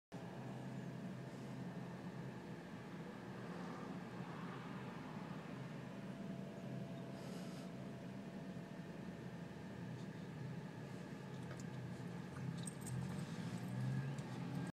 STUPID DRIVERS: Driver Leaves Insane Amount Of Space
It happens everyday. imagine the backed up traffic because of her! No, no car turned into left lane. She was just waiting there looking like a ghost bus was in front of her. It wasn't raining, sleeting, snowing, hailing, there is NOTHING on the ground. I'm so glad I was finally able to get a video but then again it happens everyday so LOL!